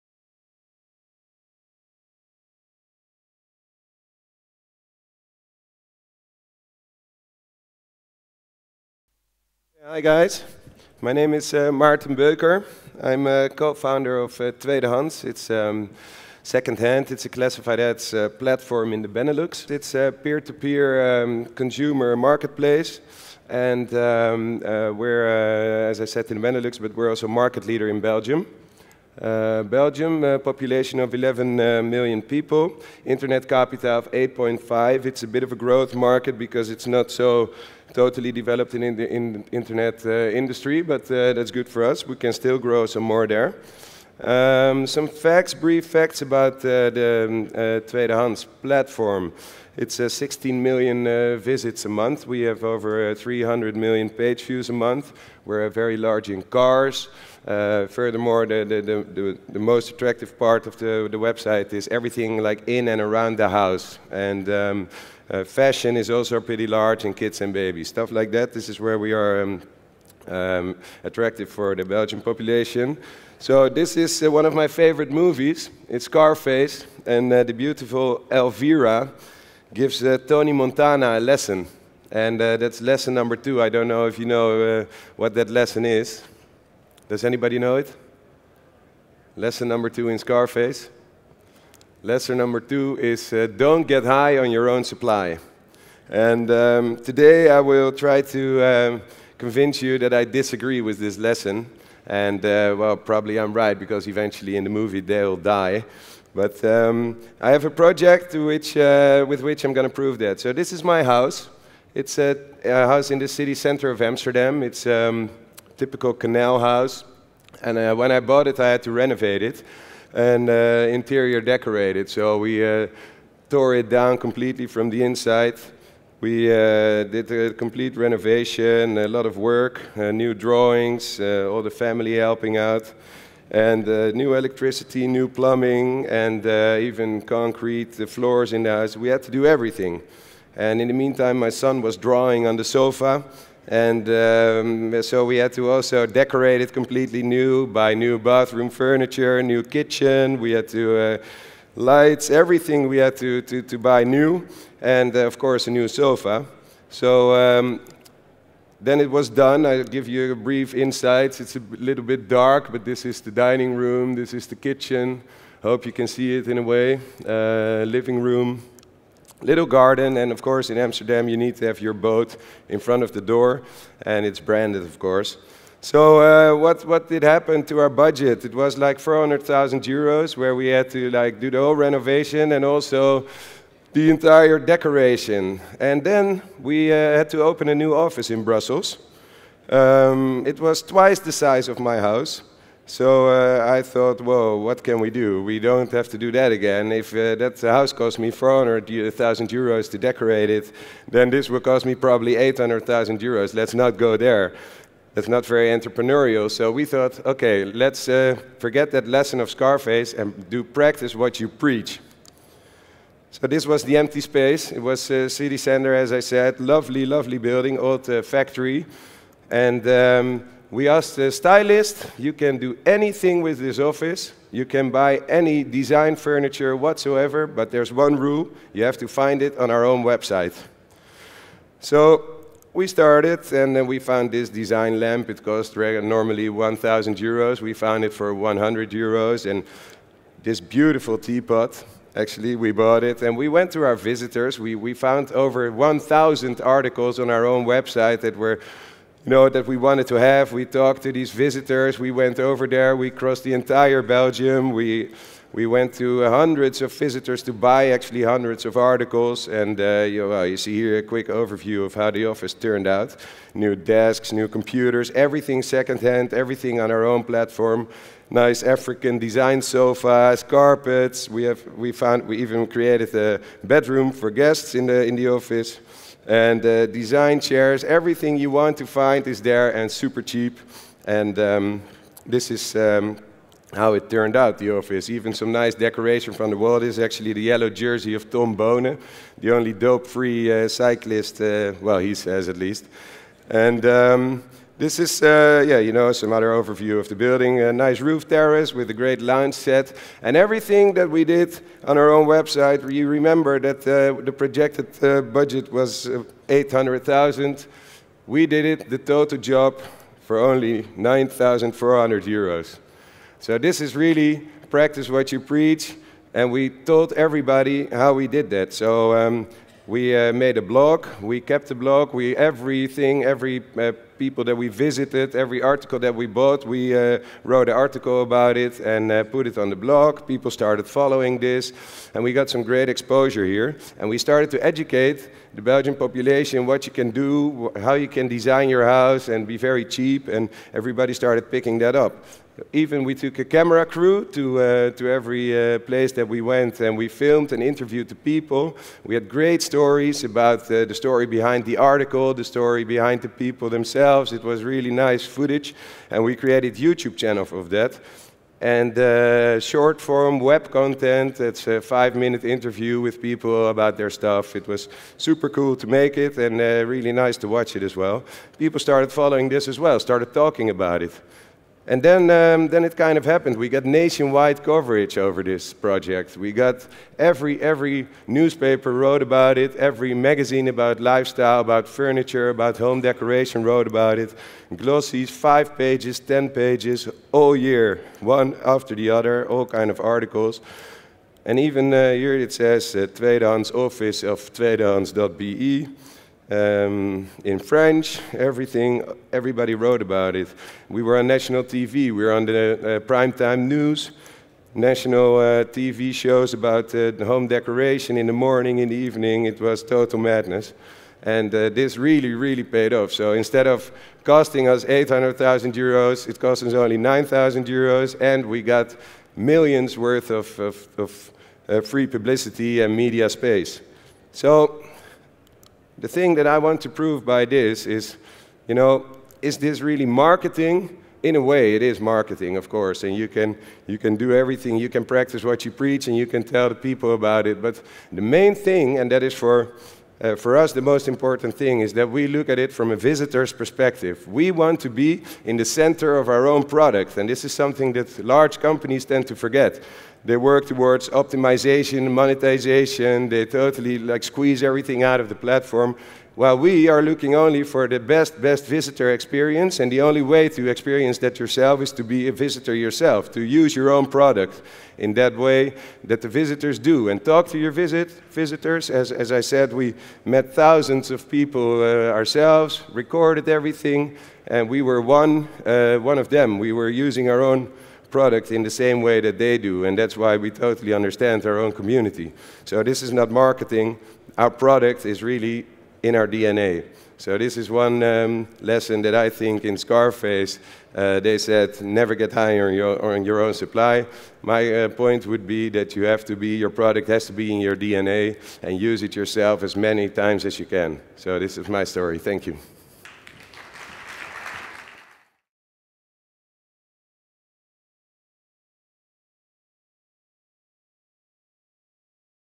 NOAH12 London - Tweedehands.be, Maarten Beucker Andreae
Rising Stars - Presentation by Maarten Beucker Andreae, Co-Founder & CEO of Tweedehands.be at the NOAH 2012 Conference in London, Old Billingsgate on the 7th of November 2012.